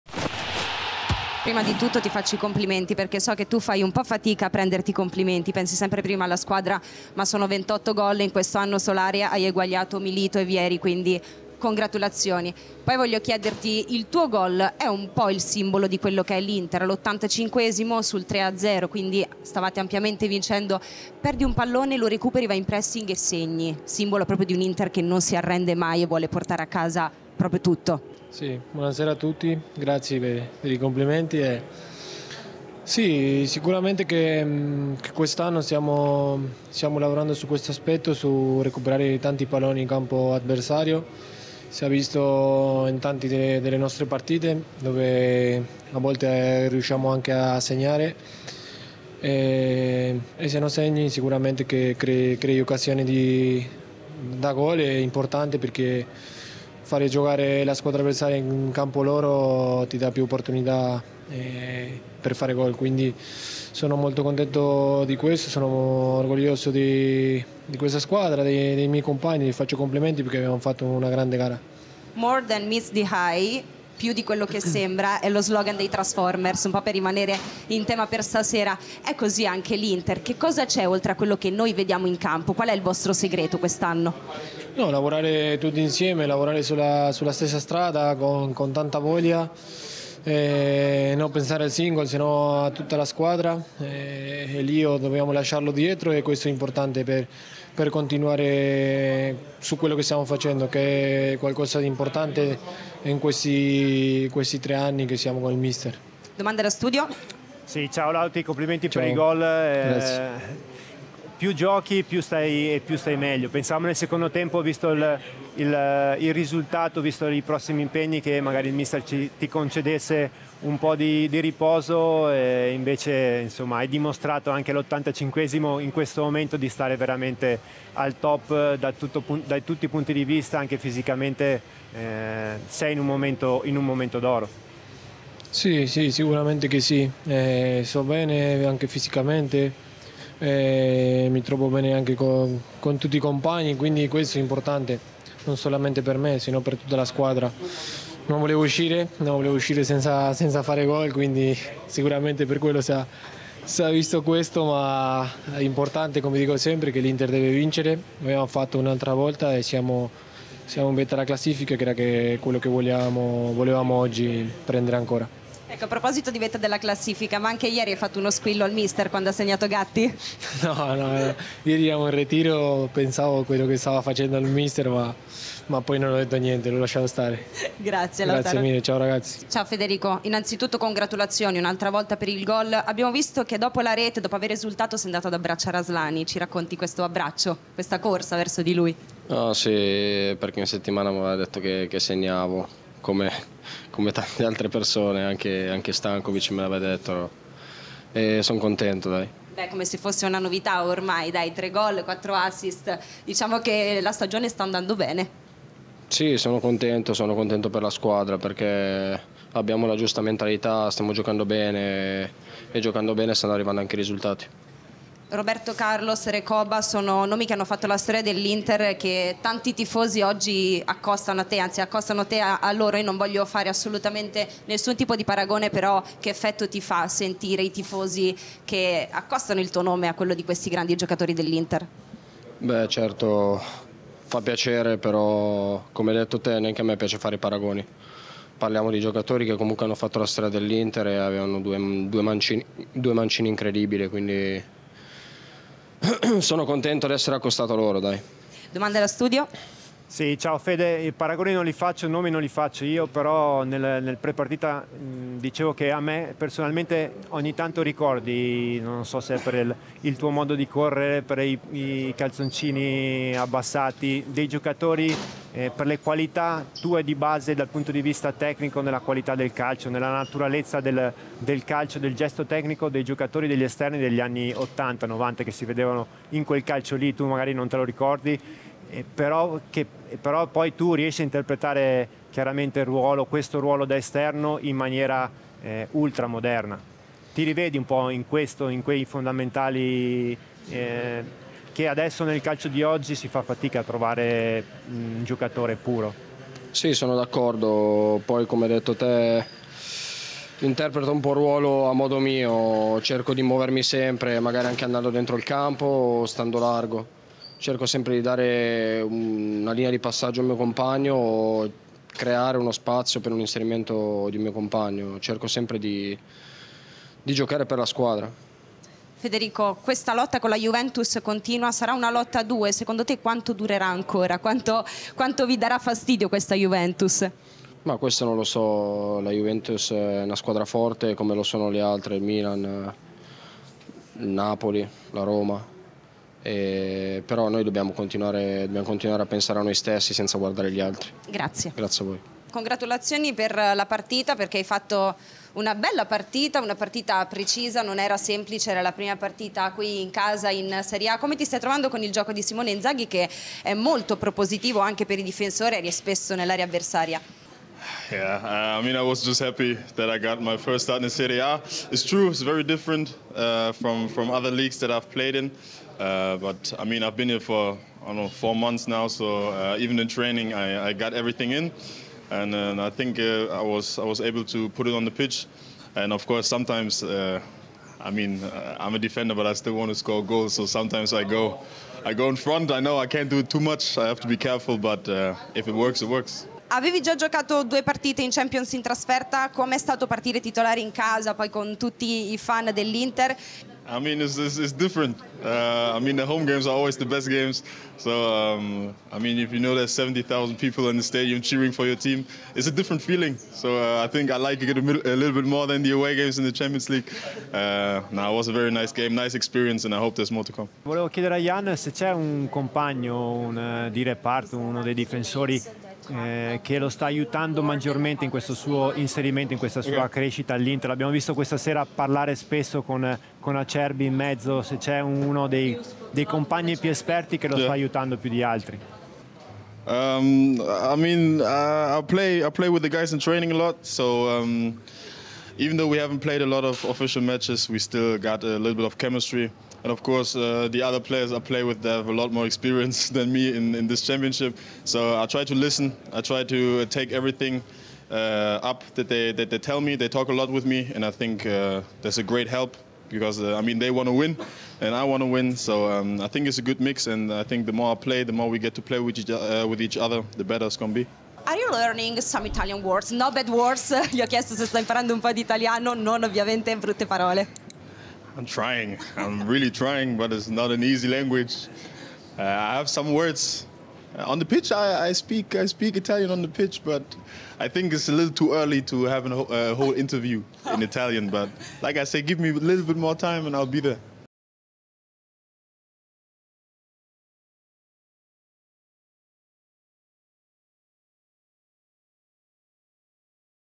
LAUTARO, DIMARCO AND BISSECK | INTER 4-0 UDINESE | PLAYERS INTERVIEW 🎙️⚫🔵
Lautaro, Dimarco and Bisseck all spoke to Inter TV after the 4-0 win over Udinese 🤩🖤💙

Lautaro, Dimarco e Bisseck hanno parlato ai microfoni di Inter TV dopo la vittoria per 4-0 contro l'Udinese 🤩🖤💙